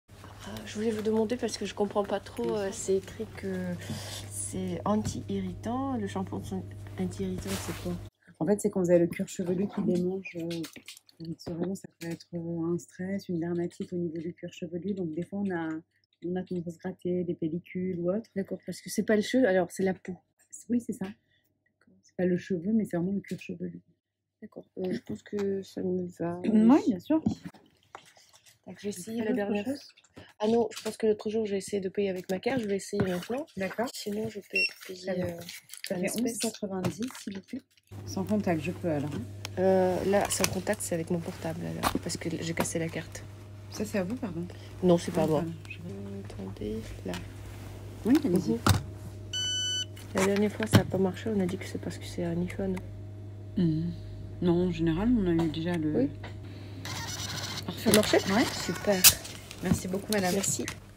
Диалог à la pharmaсie
Для наших онлайн курсов мы записываем постоянно такие диалоги.

Курсы А1 и А2 1 версии готовились в России, поэтому нам не хватало живых диалогов.

Вторые версии и В1.1 готовятся во Франции, поэтому достаточно включить диктофон и сделать запись.
Но, конечно, это иногда странно выглядит. Когда человек, вроде бы, хорошо говорит, но спрашивает очень легкие вещи или же, когда повторяет одно и тоже за собеседником)

Но, к счастью, это не все замечают, почти никто.
Вот и здесь, мы записали диалог в аптеке.

Это для уровня А2.

irritant - раздражающий (в данном контексте о коже, но может быть и в контексте эмоций: je suis irrité).

В контексте волос, когда говорят о коже головы, употребляют cuir chevelu, а не PEAU.
Sans contact - это бесконтактная оплата.

Но, конечно, есть и минусы таких записей.. это шум. Но зато максимально естественно, именно так будет в настоящей жизни, вы никогда не будете слушать кого-то, чтобы никто не перебивал, не было фонового шума и т.д.

Где еще записать диалоги и о чем поговорить?)

Вы согласны?

В1.1 (живые вебинары с 9 января 2020)

Хорошего дня!